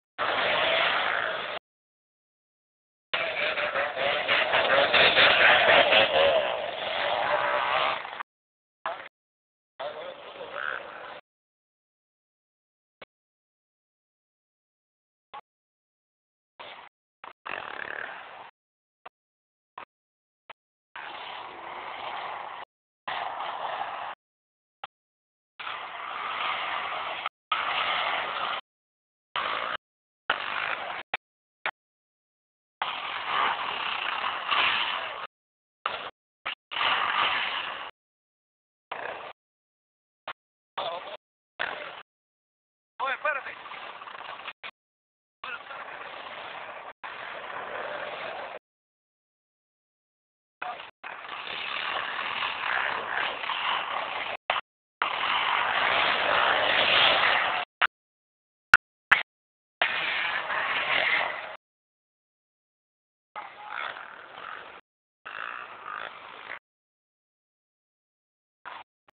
ridin in white rock miami 305
ridin in white rock this place is atv heaven